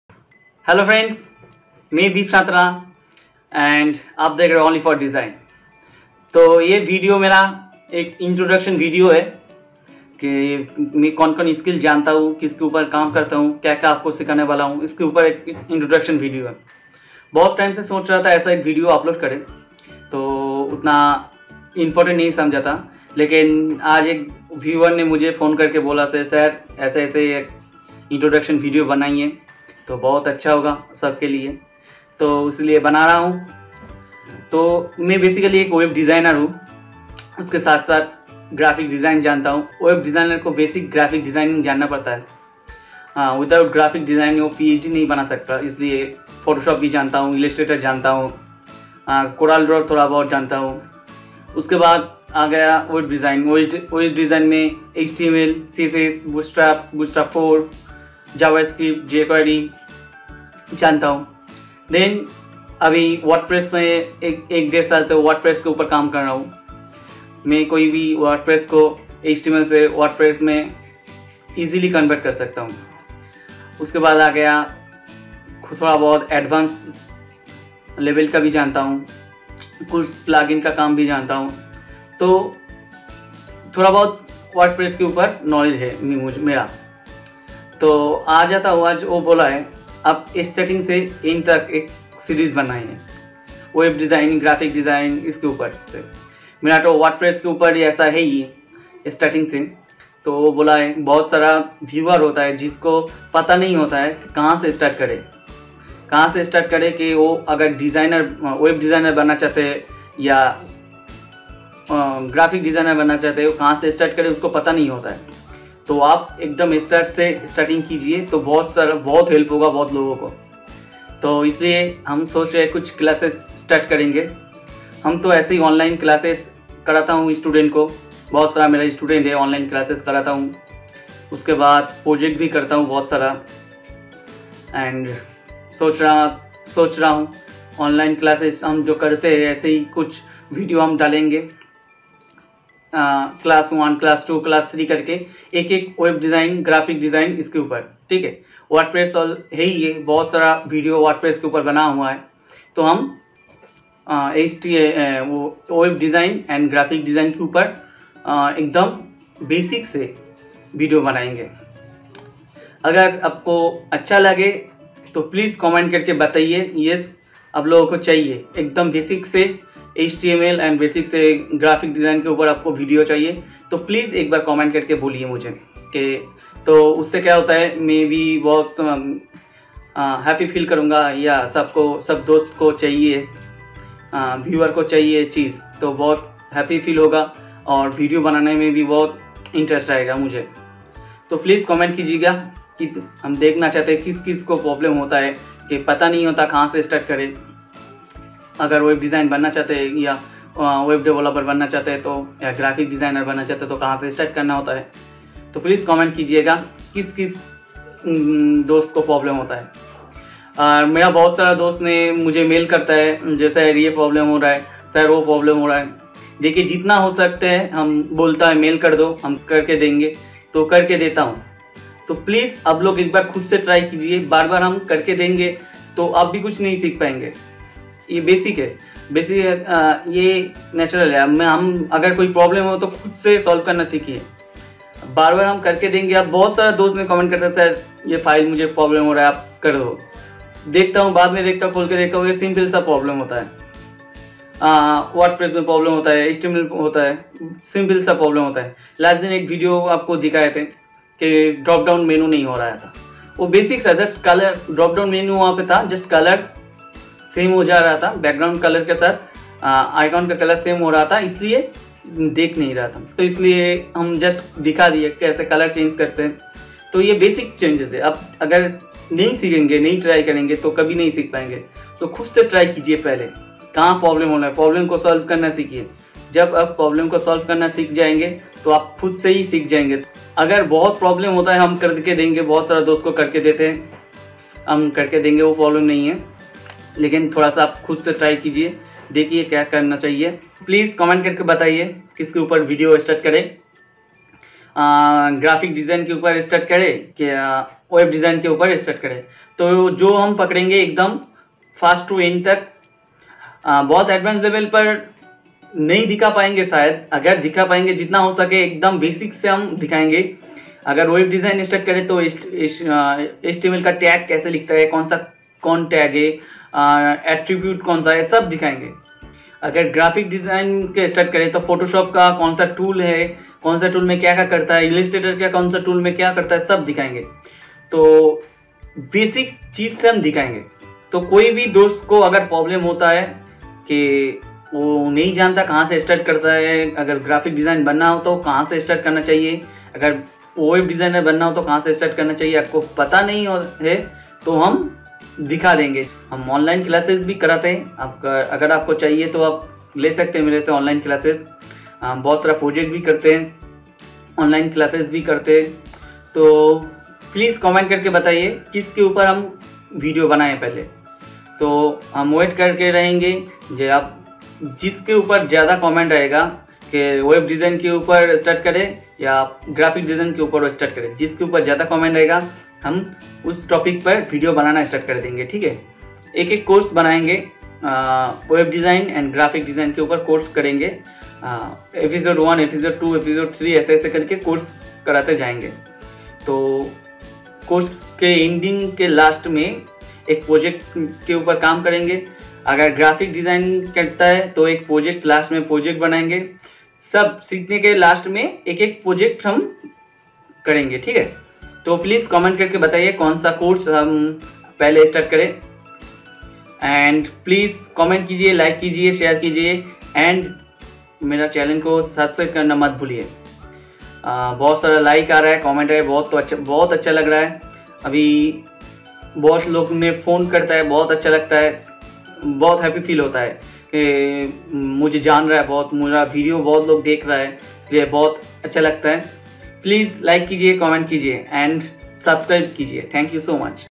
Only for Designs Channel Guide and Introduction - onlyfordesigns
Hey Everyone, welcome to Only for Designs. This video is a guide for you to get the maximum value out of this channel.

My name is Dip and I teach Graphic Design, Web Design, WordPress Development and many other things related to technology.

You can learn many language in a systematic sequence which is created in form of PLAYLISTS on this channel. Every video is so detailed that you might not need to ask a single question. Still if you want to ask any question, you can ask that in same video's comment box. I will definitely reply your query if you are a subscriber and if I can see that.

My name is Dip Santra, and I am a seasoned Web Design Trainer having experience of 4+ years. I've started this training for anyone who wants to learn Graphic Designing, Web Designing and WordPress for FREE in a professional way.

Here's the complete playlist of WordPress Training so far, I suggest, if you are new to WordPress you should watch whole playlist from the beginning: